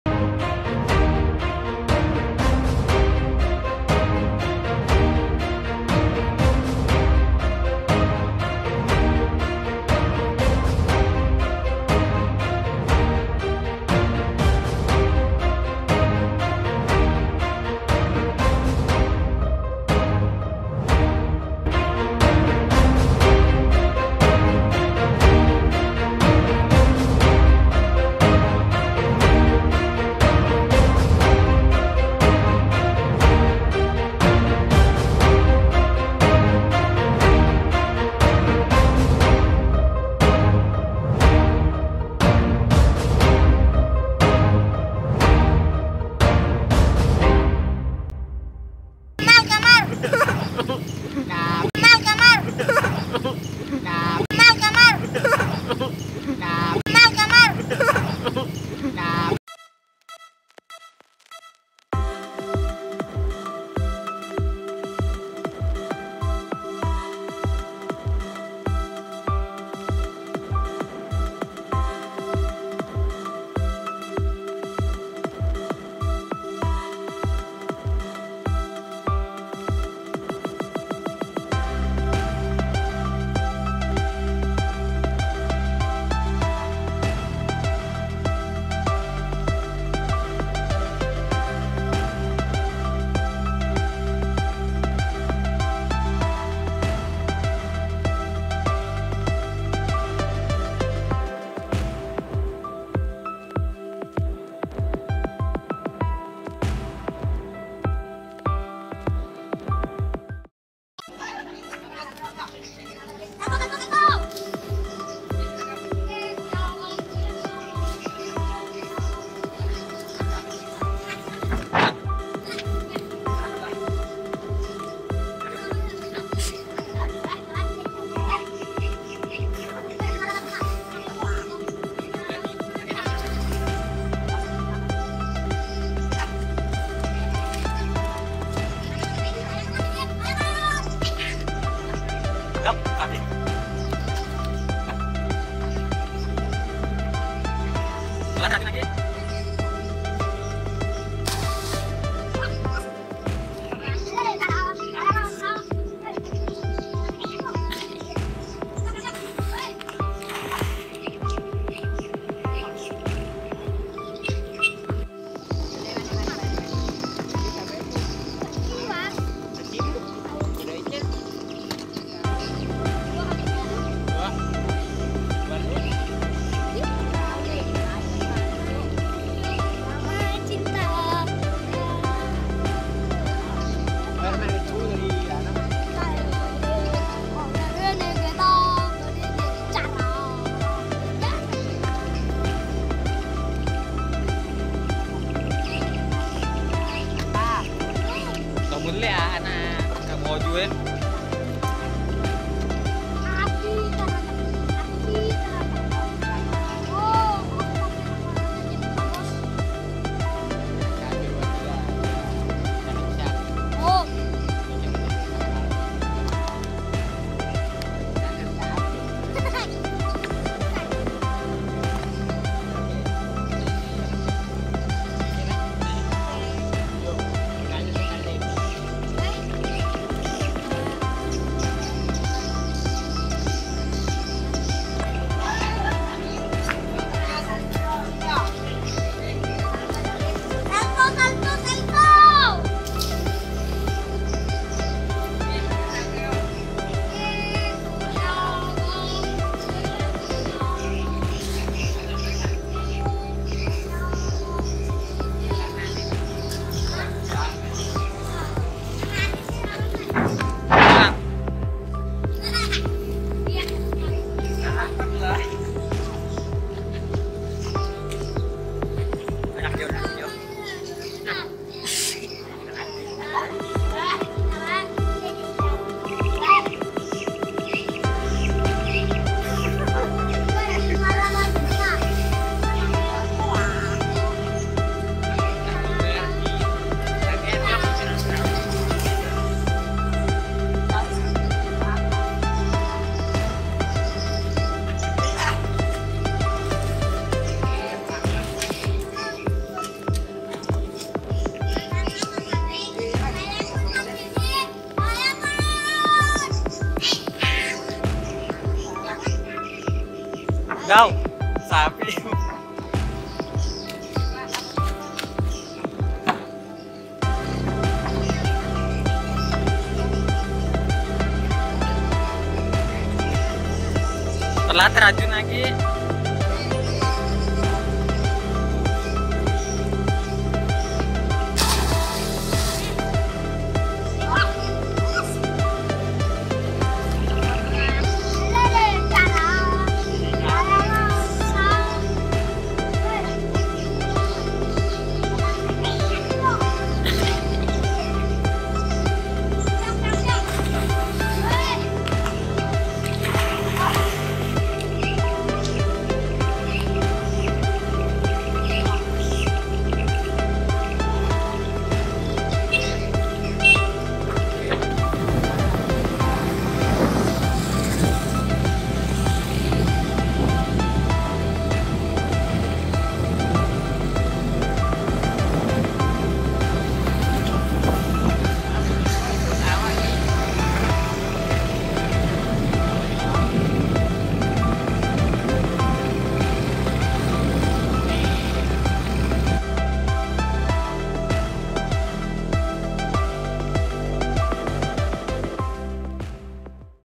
Bocil Mandi di Sungai
Video ini di buat Mematuhi Children's Online Privacy Protection Act
( COPPA)
video ini bukan konten pornografi anak
video ini Hanya tangkapan Kesenangan Anak Anak saat melepas lelah.
setelah beraktifitas : Latihan Bola
dan mereka menceburkan dirinya ke sungai.